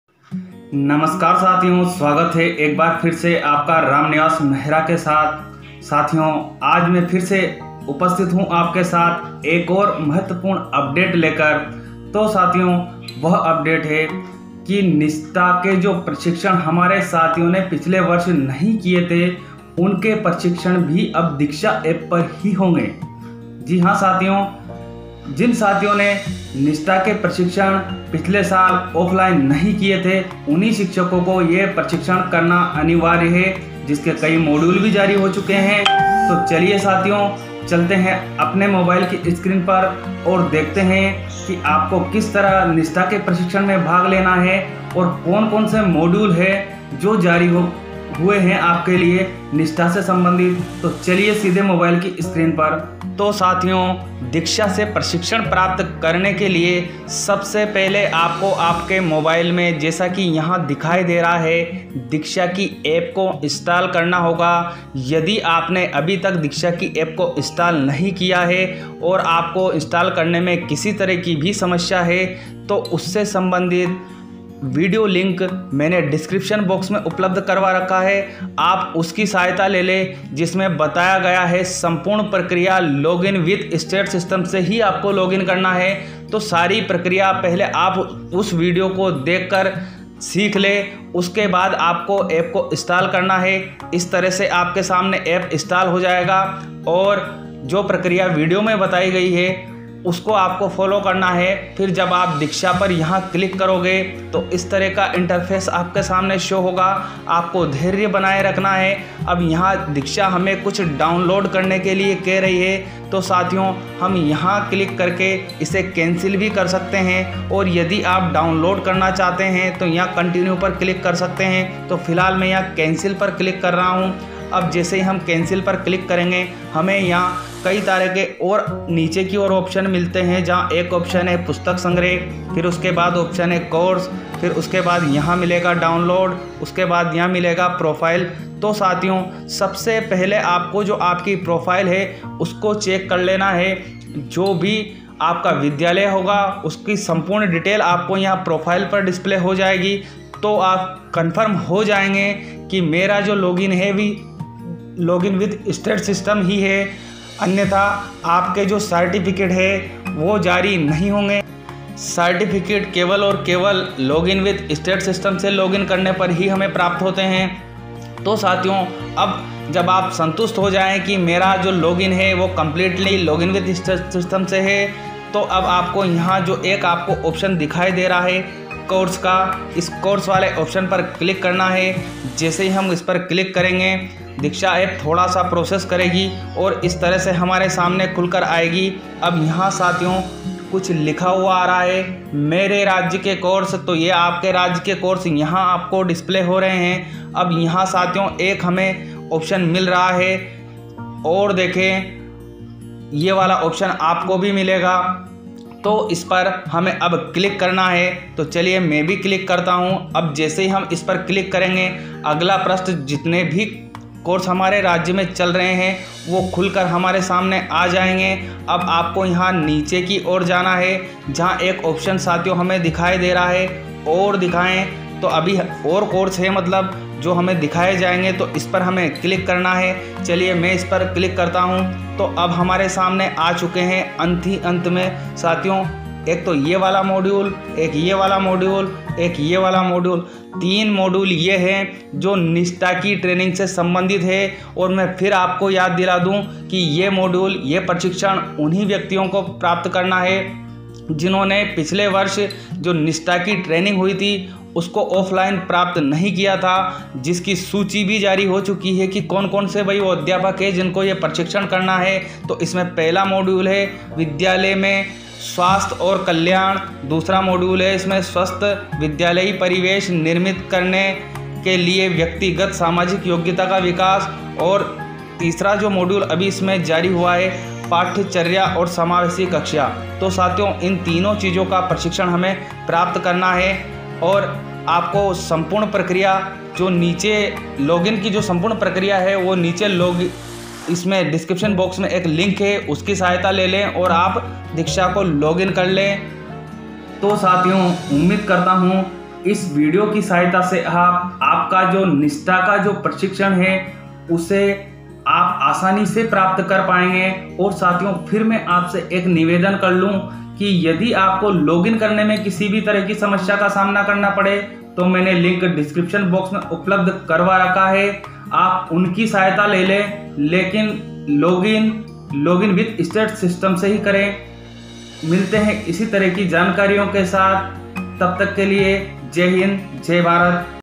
दीक्षा से निष्ठा प्रशिक्षण में भाग कैसे ले Diksha se nishtha training online kaise le nishtha traing
🙏🏾 *प्रशिक्षण अपडेट*🙏🏾
*साथियों जैसा कि हम सबको विदित है कि हमारे 1 से 5 एवं 6 से 8 के प्रशिक्षण ऑनलाइन चल रहे हैं तो प्रस्तुत है आपकी सहायतार्थ समस्त वीडियो*

 आपको उक्त प्रशिक्षणों को प्राप्त करने में यदि समस्या हो तो आप दिए गए *लिंक पर क्लिक करके वीडियो की सहायता* ले सकते हैं
*धन्यवाद*